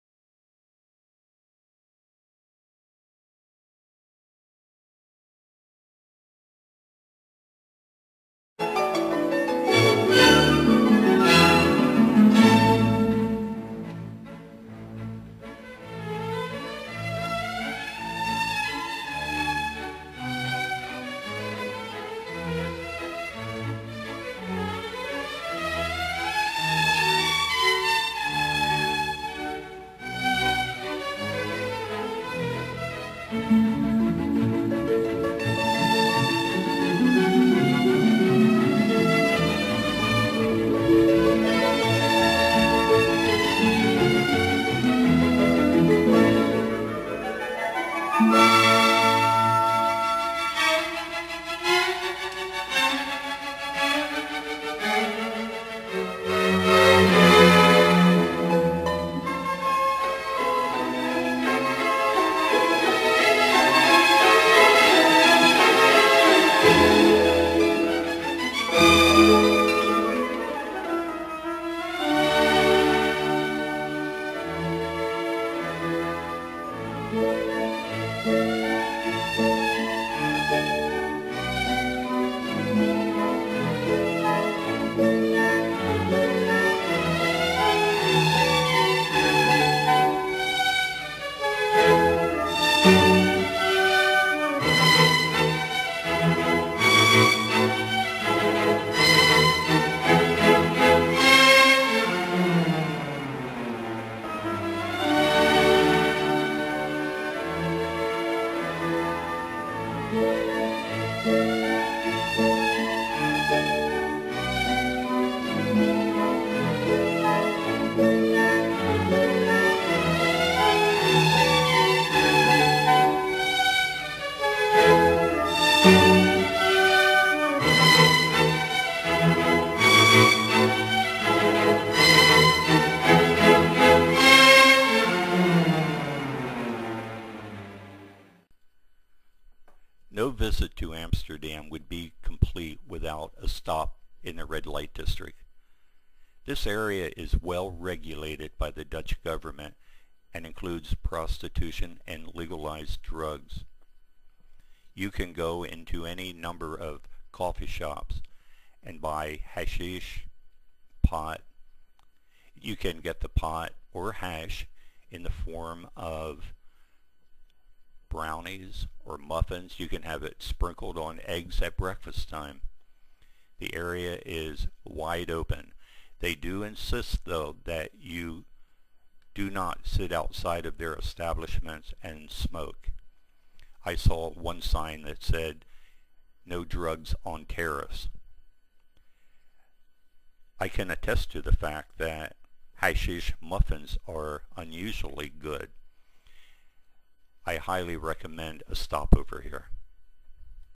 016 - 2007 Trip - Amsterdam - Red Light District
Scenes of the Red Light District of Amsterdam